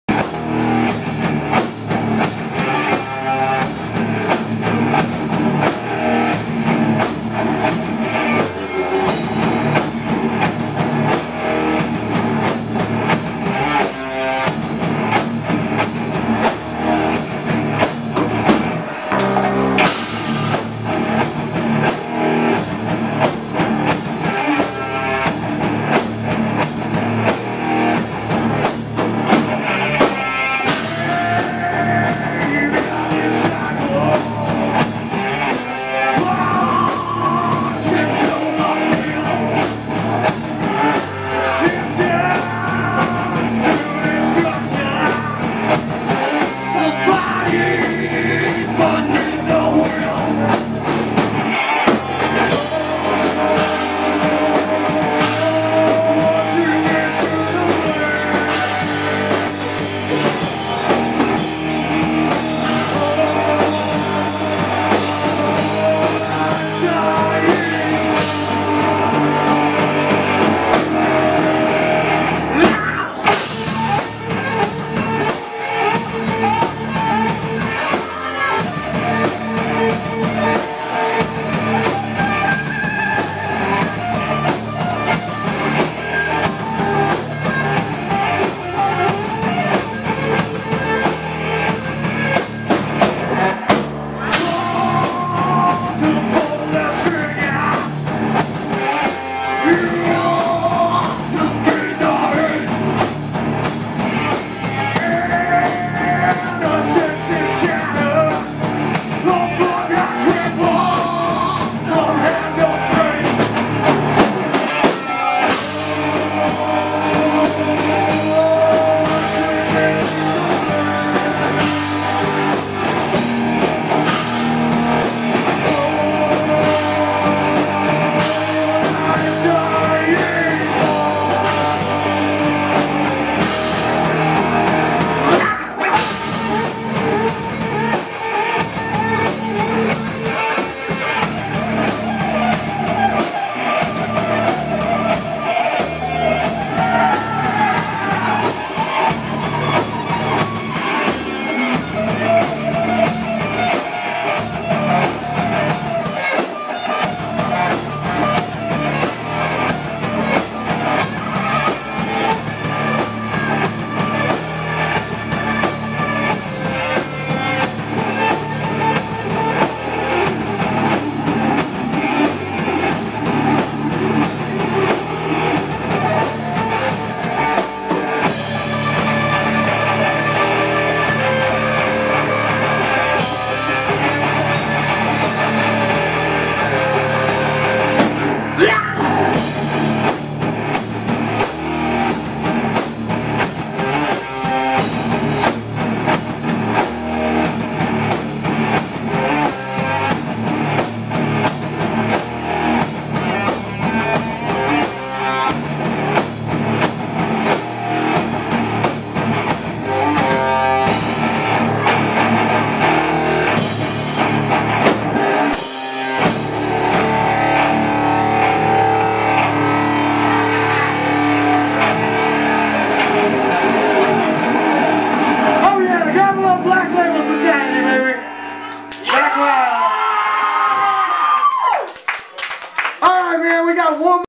Caducus 12/03/2011 Dixie Creek
Caducus performing at the Dixie Creek Saloon in Tangent, OR